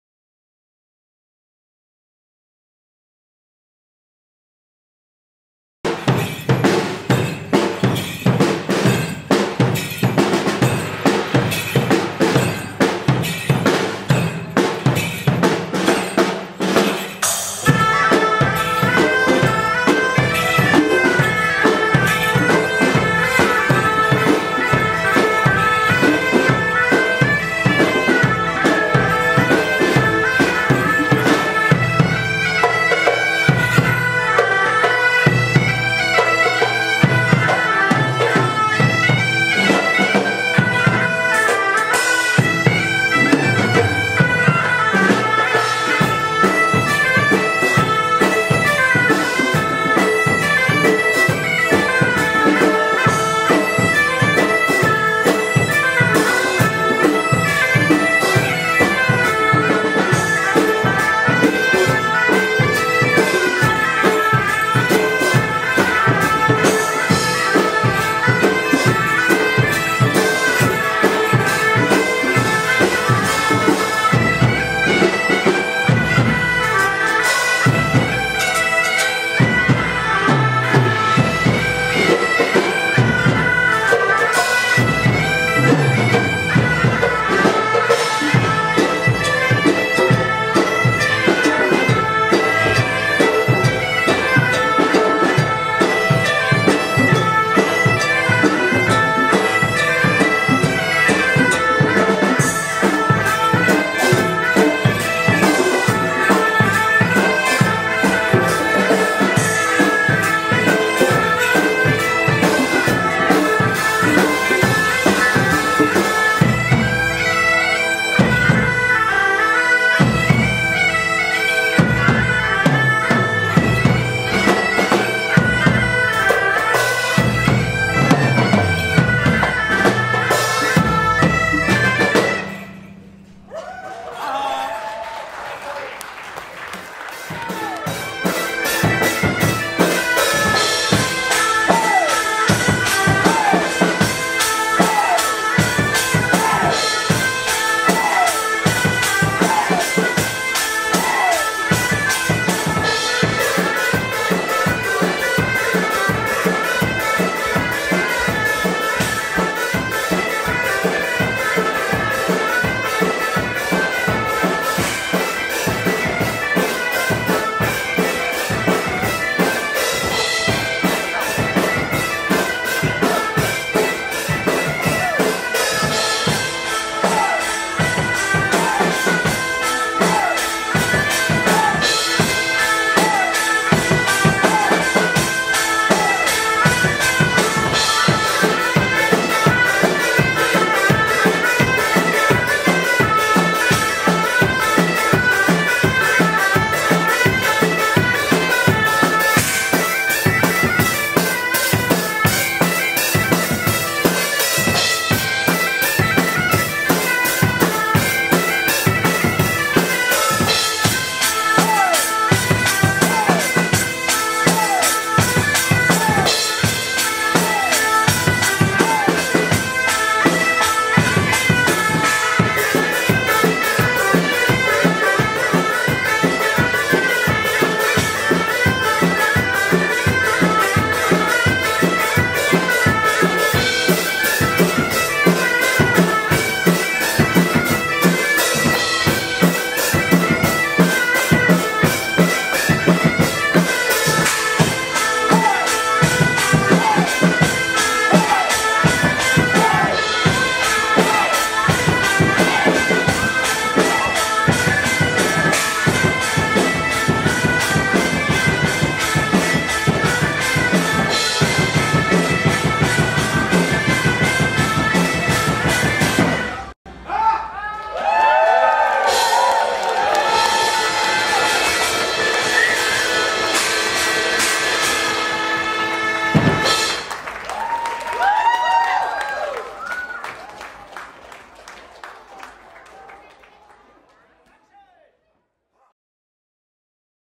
Die Vertriebenen auf dem Mittelalterfest zu Wedel 2019
Die Vertriebenen beglückten das zufriedene Publikum auf dem Schlangenschrein Mittelalterfest zu Wedel 2019...auch Markus Emerich scheint zufrieden...